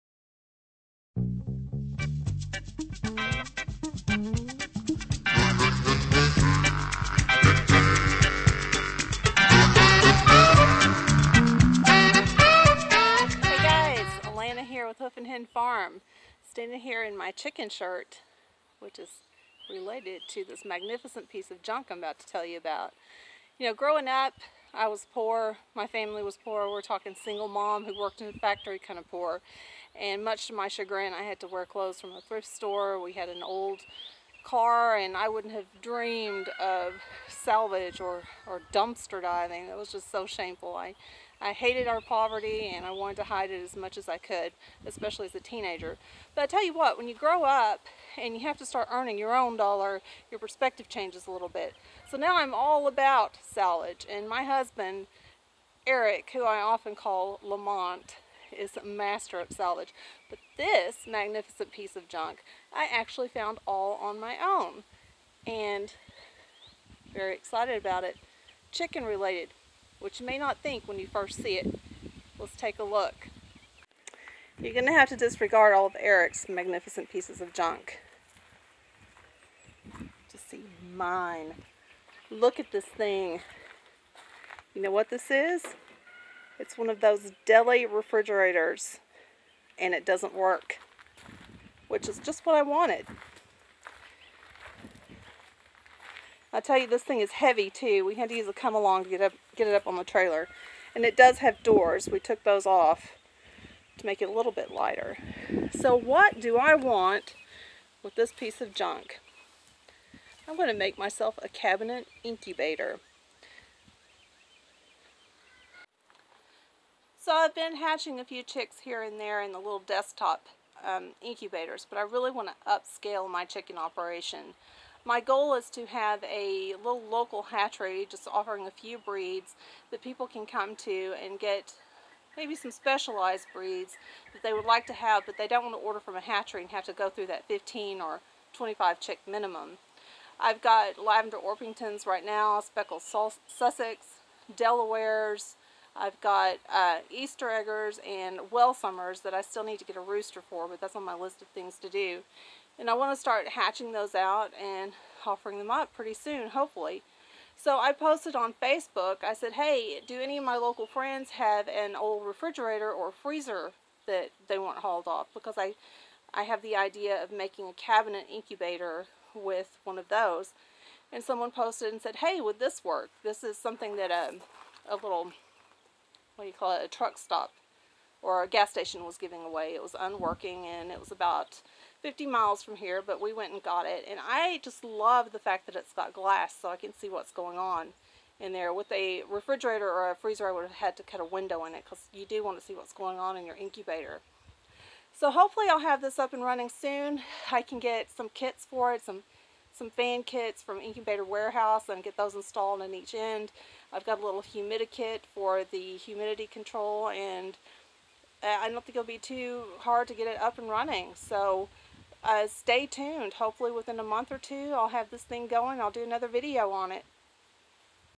DIY Cabinet incubator: The first step!
The first step in building your own cabinet incubator is acquiring a suitable cabinet.  We are going to turn this magnificent piece of free junk into an awesome incubator.  We will be following up with videos on the actual implementation.